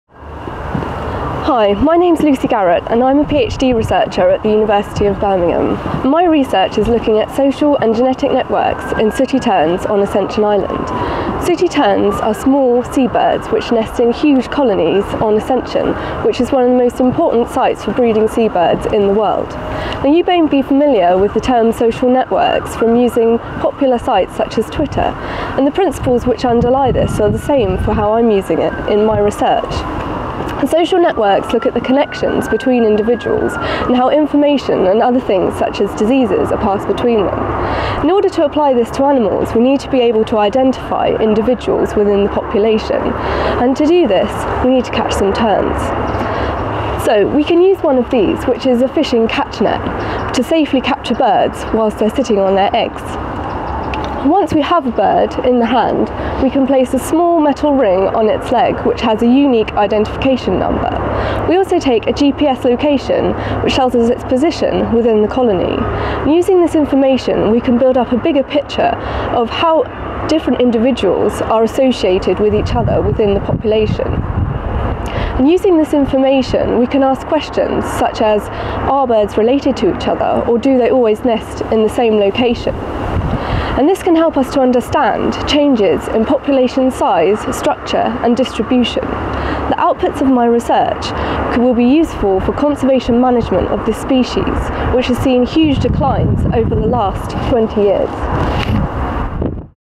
Researching Seabird Social Networks
In this short video Lucy Garrett presents her research into Seabird Social Networks, in an attempt to further our understanding of seabird ecology and colonial behaviour in a population of Sooty Terns on Ascension Island.

Lucy is conducting her research as part of her PhD with the AOS (Army Ornithological Society) at The University of Birmingham.

You can follow Lucy on twitter for research updates: (@garrettlucy1)

The video was produced as part of an Engaging Research training programme. Funded by a NERC Innovation Award, the training was coordinated through the Central England NERC Training Alliance (CENTA) Doctoral Training Programme